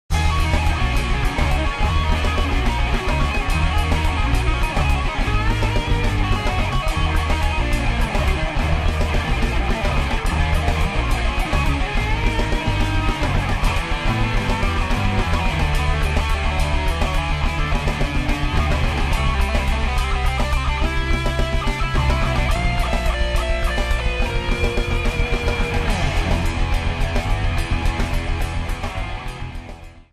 SkinYourSkunk.com Guitar Skins TV Commercial
Premium guitar skins for any size, shape or style...guitar, bass or acoustic.  We carry over 1500 designs made with the best materials available.  Visit us at SkinYourSkunk.com today.